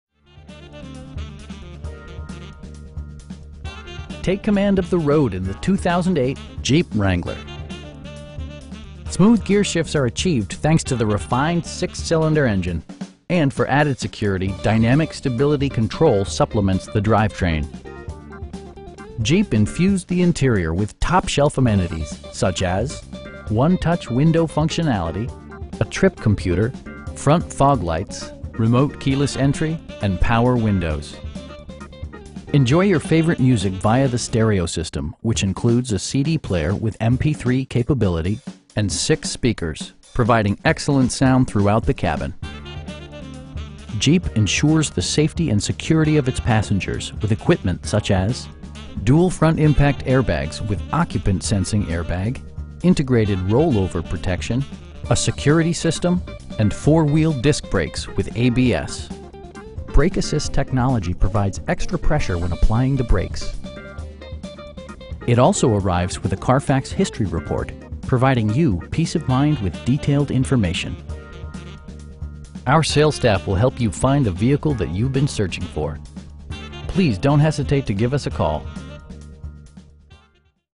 2008 Jeep Wrangler Unlimited Sahara in Austin, TX 78758
Take command of the road in the 2008 Jeep Wrangler. Smooth gearshifts are achieved thanks to the refined 6 cylinder engine, and for added security, dynamic Stability Control supplements the drivetrain. Four wheel drive allows you to go places you've only imagined. Jeep infused the interior with top shelf amenities, such as: 1-touch window functionality, a trip computer, air conditioning, front fog lights, remote keyless entry, cruise control, and power windows. Jeep ensures the safety and security of its passengers with equipment such as: dual front impact airbags with occupant sensing airbag, integrated roll-over protection, traction control, a security system, and 4 wheel disc brakes with ABS. Brake assist technology provides extra pressure when applying the brakes. It also arrives with a Carfax history report, providing you peace of mind with detailed information. Our sales staff will help you find the vehicle that you've been searching for. We'd be happy to answer any questions that you may have. Please don't hesitate to give us a call.